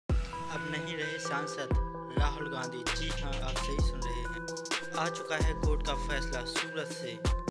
अब संसद नही रहे राहुल गांधी कोर्ट ने दिया फैसला
Rahul Gandhi defamation case में राहुल गांधी की सदस्यता रद्द होने वाली है ?
लोकसभा के स्पीकर राहुल गांधी की सदस्यता अपील से पहले ही रद्द कर सकते हैं ?